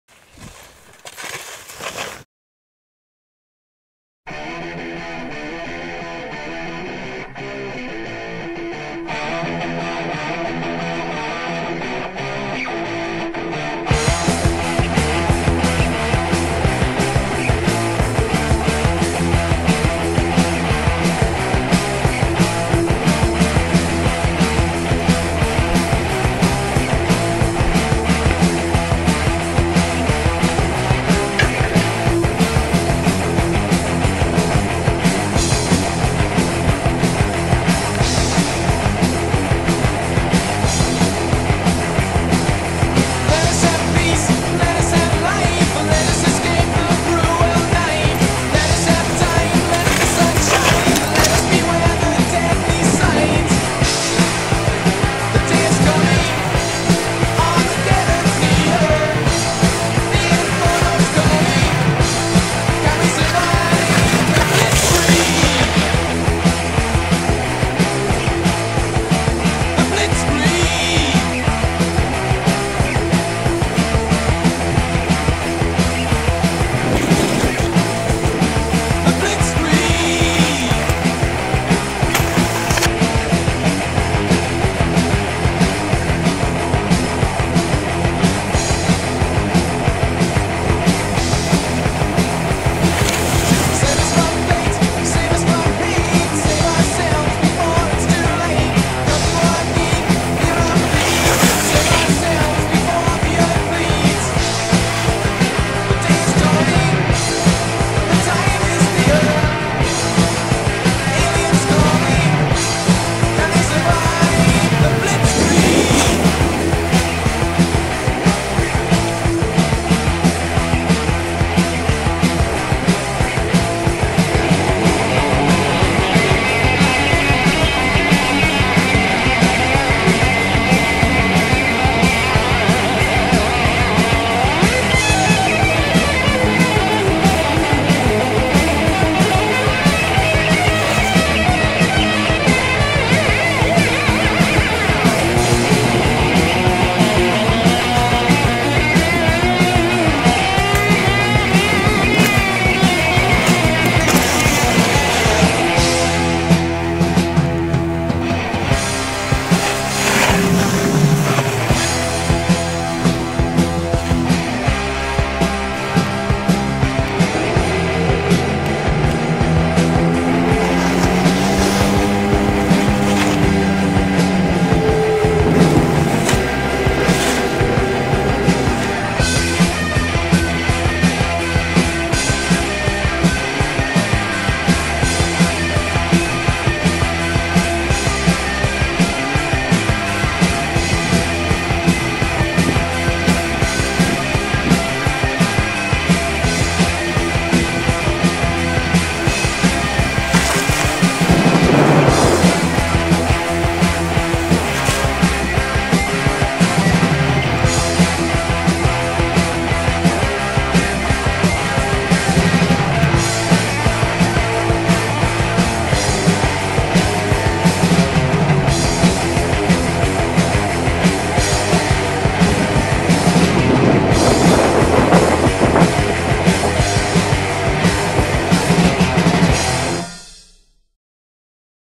DRIFTER SNOWBIKES Rigaud Resort, Quebec
DSB owners Drifter Dave and Mavrick hit the road to Montreal for a fun session through Rigaud's unique mogul runs and treelines. Watch those board angles and see how SLAY kits handle the roughest terrain with ease!